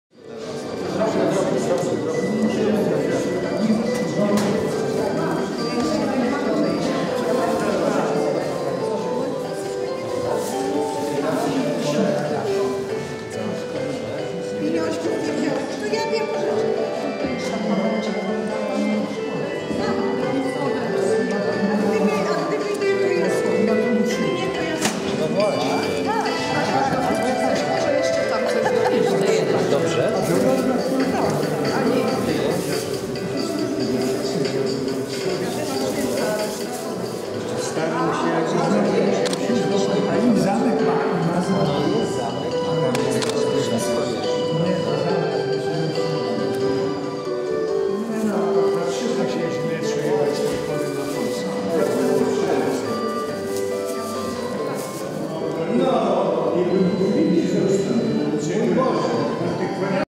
Promocja „Kalendarza Olsztyna 2013”
Promocja XV edycji „Kalendarza Olsztyna” na rok 2013 | Zamek Kapituły Warmińskiej w Olsztynie | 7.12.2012